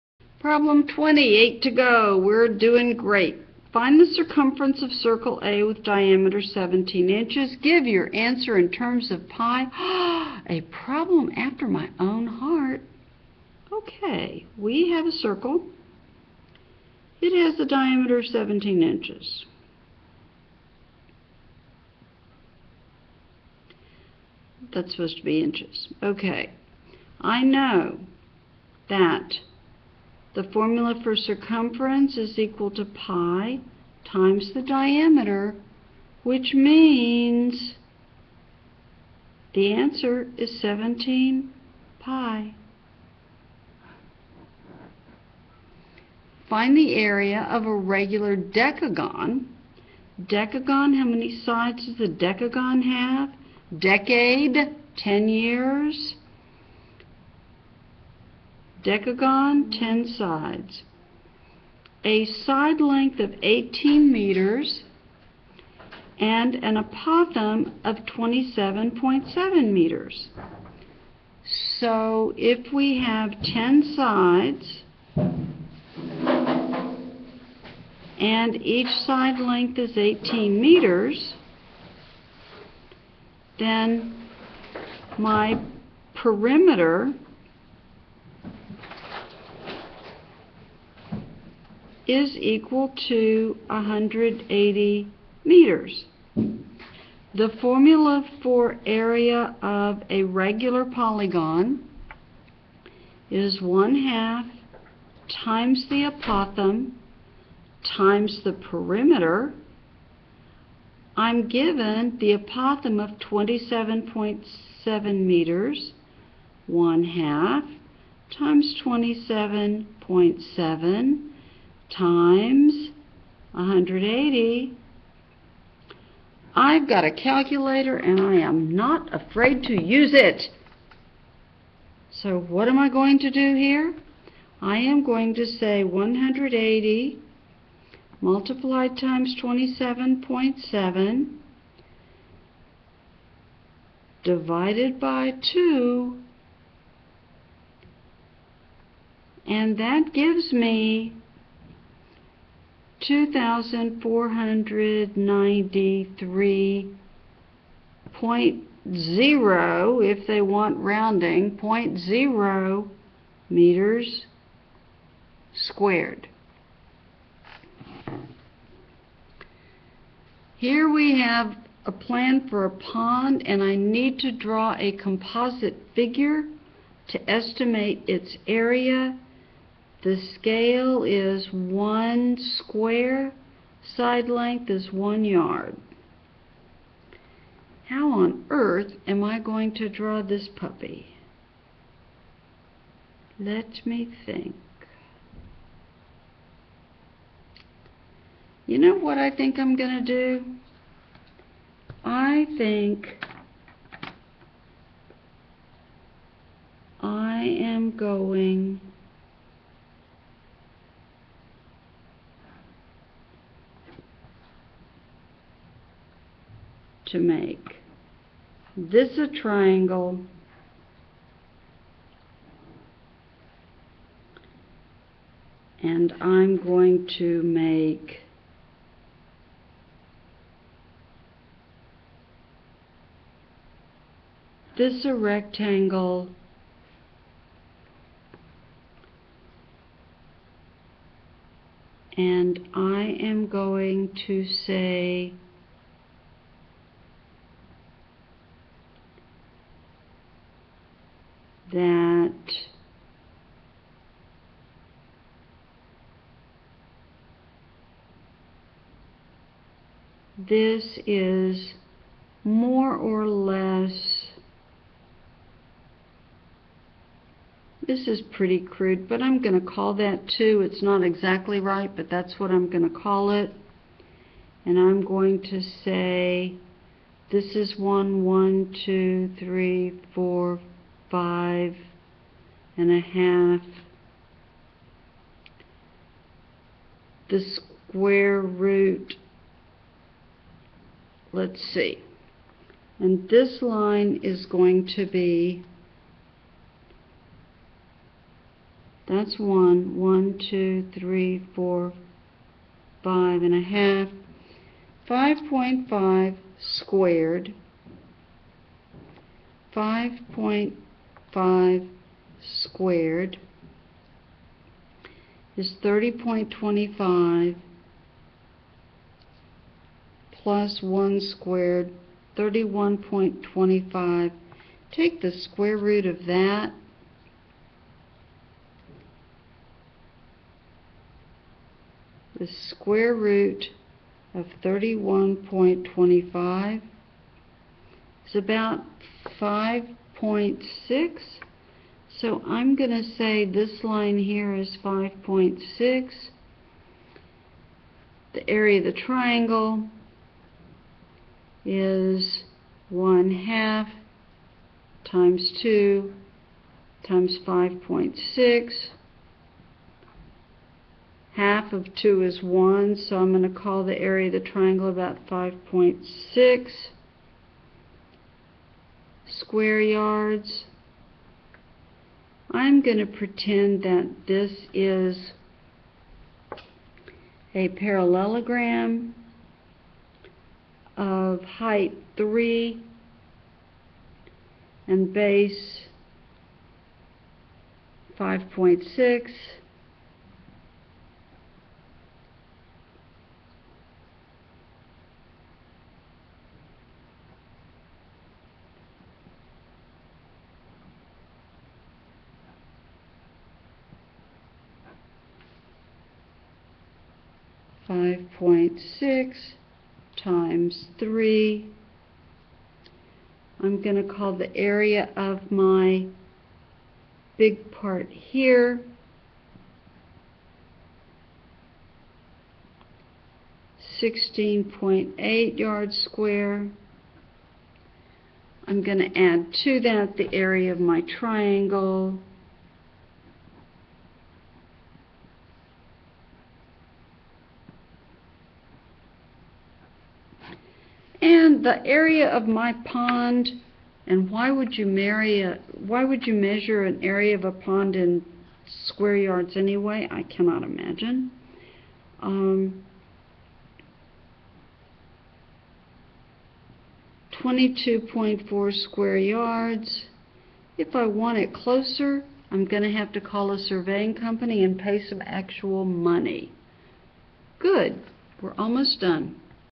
Unit 11 20 - 22 Easy word problems involving area of polygons
A coordinate grid is superimposed over a pond. Calculate the surface area of the pond. Calculate the area of a regular decagon with side length of 18 m and apothem of 27.7 meters. Given the diameter, calculate the circumference of a circle.